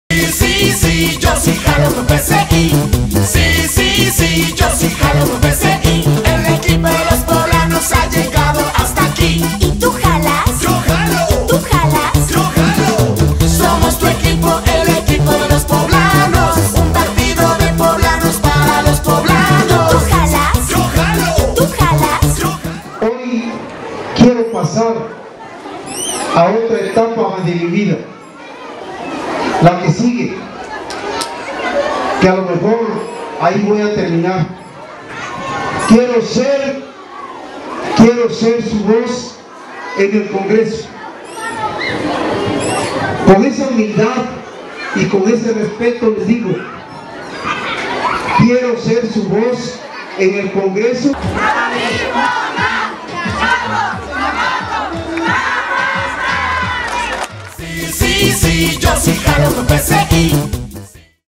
Arranque de campaña en Palmar de Bravo
30 de abril 2018 - Día del Niño

Nuestros candidatos Carlos Navarro como diputado al Dtto.15 y Jacobo Román, a la presidencia municipal de Palmar de Bravo, celebraron a las y los niños de este municipio en su día.

Y TÚ, ¿jalas?